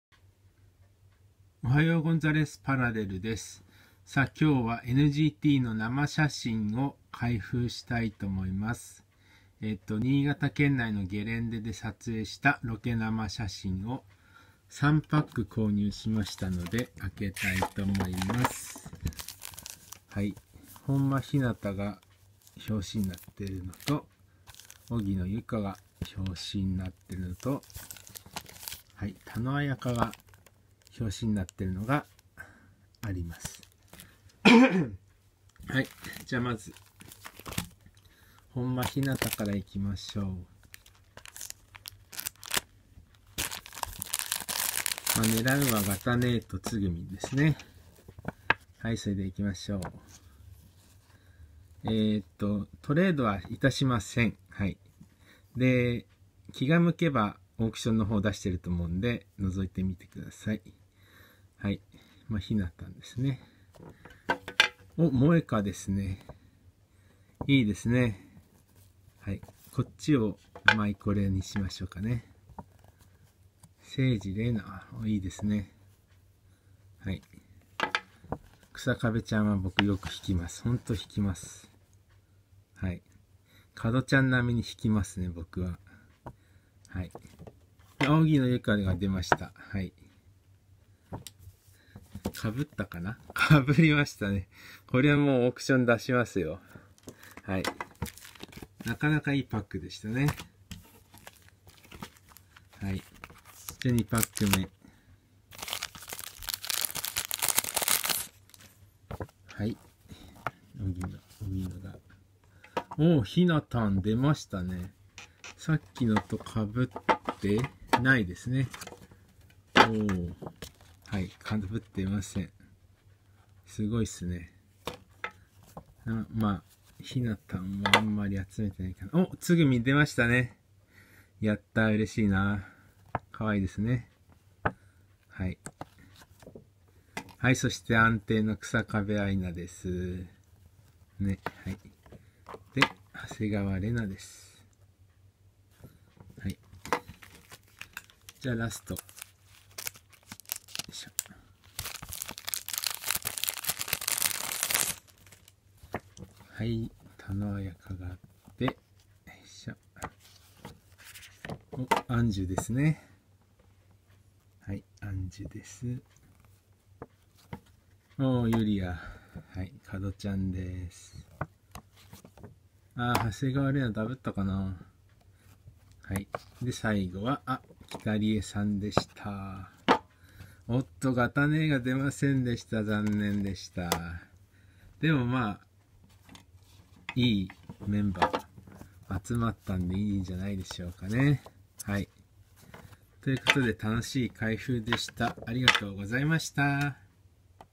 【開封】NGT48 2018 1月ロケ生写真 ゲレンデ
NGT482018年1月ランダム生写真の開封です。メンバーが寒い中ロケしたショット満載！